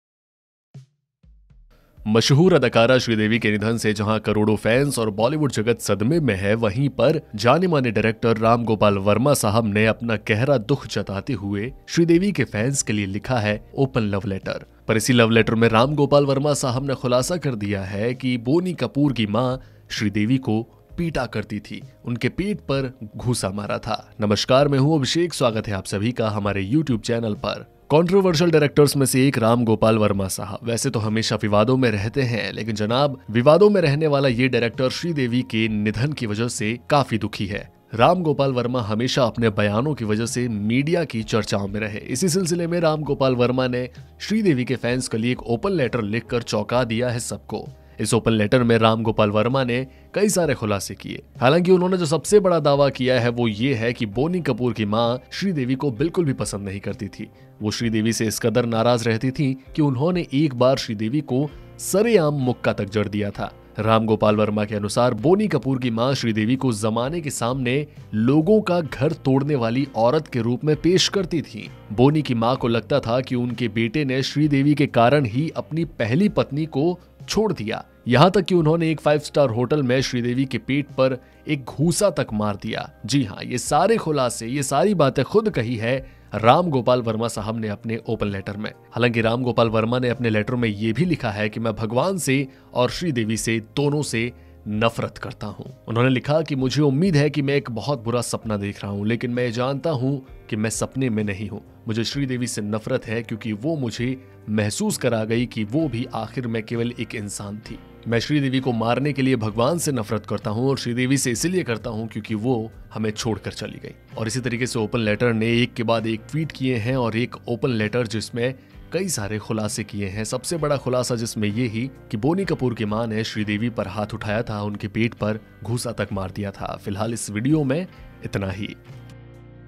Sridevi Punched by Boney Kapoor's mother, when she was pregnant, says Ram Gopal Varma | Boldsky
Ram Gopal Varma has written a "love letter" to Sridevi's fans on Facebook. In this latter he reveals many unknown incidents of Sridevi's life. Ram Gopal Varma writes, "Boney 's mother portrayed her as a home breaker and publicly punched her in the stomach in a five star hotel lobby for what she did to Boney's first wife Mona."

रामगोपाल वर्मा ने श्रीदेवी के फैंस के लिए एक ओपन लेटर लिखकर चौंका दिया है। बड़ा खुलासा  करते हुए उन्होंने लिखा है की बोनी कपूर की मां श्रीदेवी को बिल्कुल पसंद नहीं करती थीं। वो श्रीदेवी से इस कदर नाराज रहती थीं कि उन्होंने एक बार श्रीदेवी को सरेआम मुक्का तक जड़ दिया था।